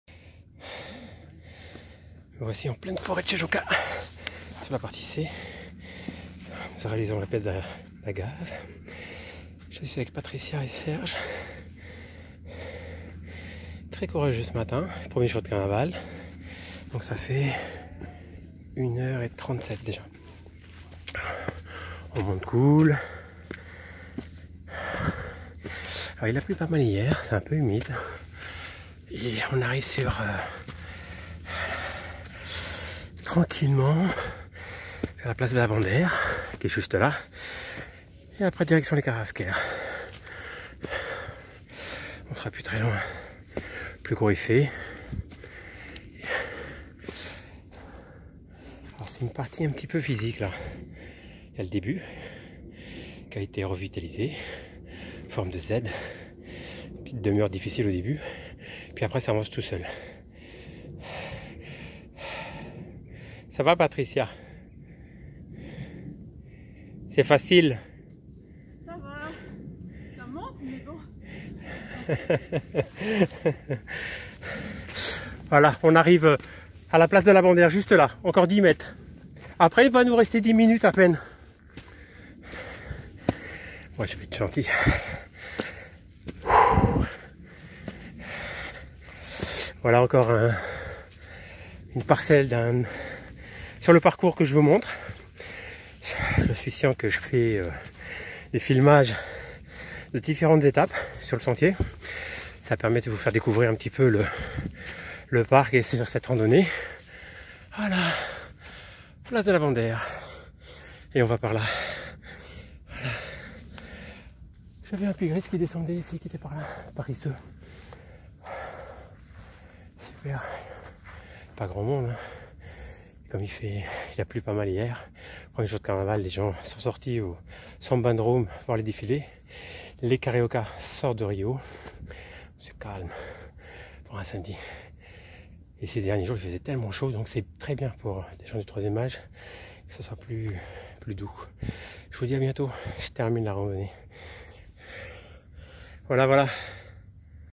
Randonnée forêt de Tijuca (Pedra da Gavea)
La randonnée la Pedra da Gavea dans la forêt de Tijuca est une des plus physiques. Cependant, pour les amateurs de marches, elle reste accessible pour des enfants et même les personnes du troisième âge.
Si vous souhaitez réaliser une randonnée à Rio, je me ferai un plaisir de vous faire découvrir la forêt de Tijuca.

Trekking ou randonnée à la Pedra da Gávea, est le sentier le plus hard de toutes les randonnées de Rio. Le reste à côté, est incomparable. La difficulté physique est t'el qu'après seulement les 30 à 45 premières minutes nous croissons la plupart du temps des touristes qui retournent en bas, avant même d'avoir pu atteindre le sommet. Marcher à travers les racines des arbres avec un dénivelé important, épuise rapidement les marcheurs. Seules ceux qui auront un courage à toutes épreuves pourront franchir se cape et terminer jusqu'au bout cette randonnée.